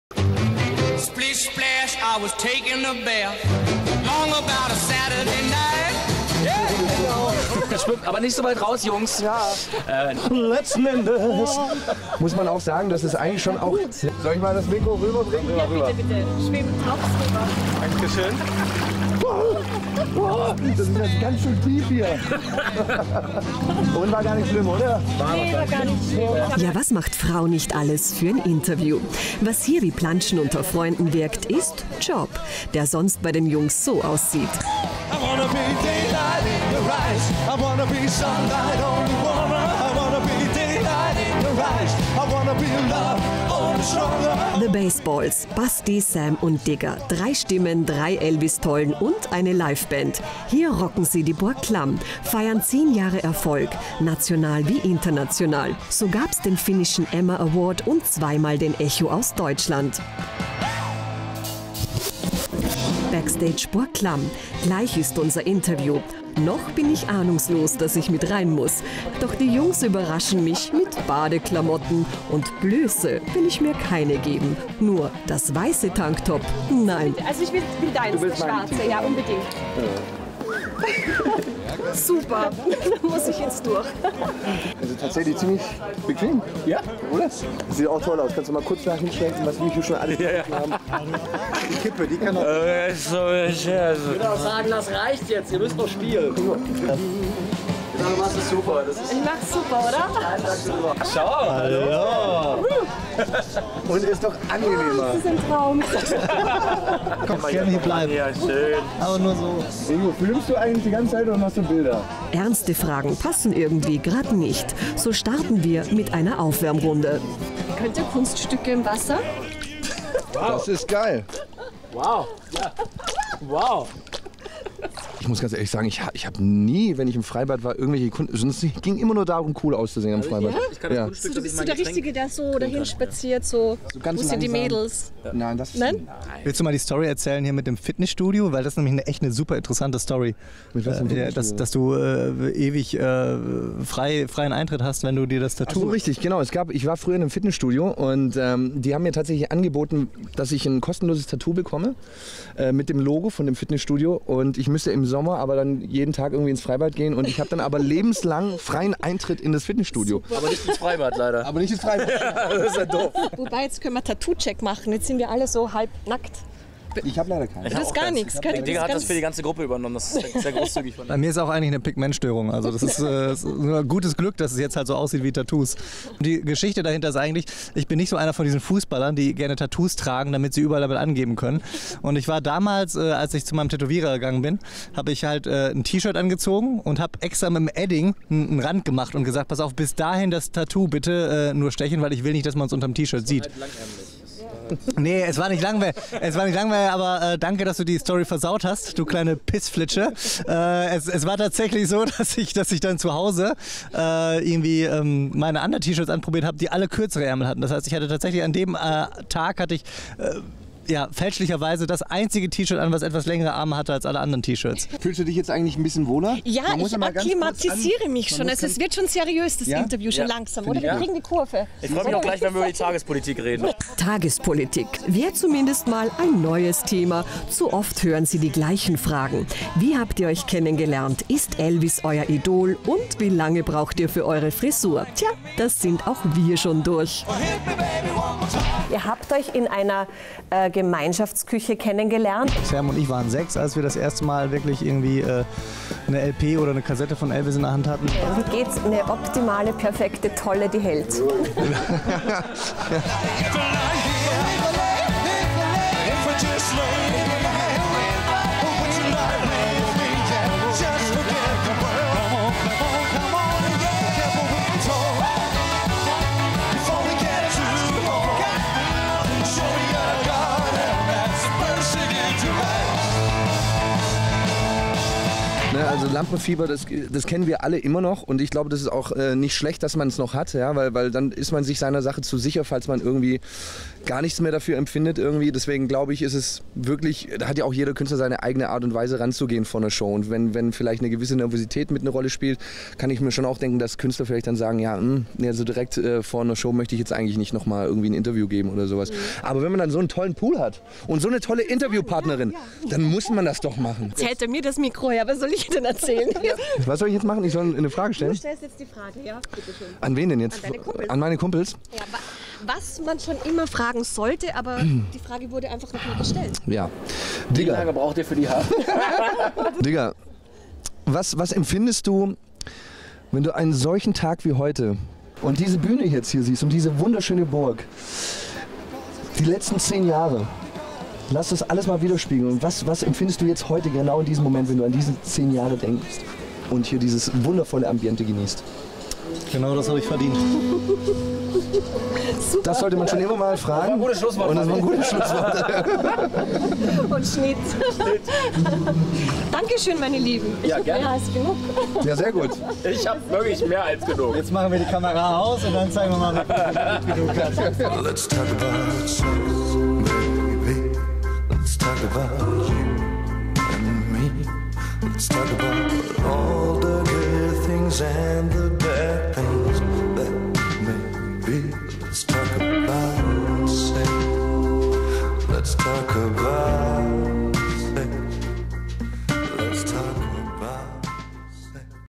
The Baseballs "plantschen" mit Petra Stumpf
"Poolparty" mit The Baseballs auf Burg Clam I
TV-Talk mit Petra Stumpf für LT1 I
Wenn das Interview überraschend im Pool landet ;)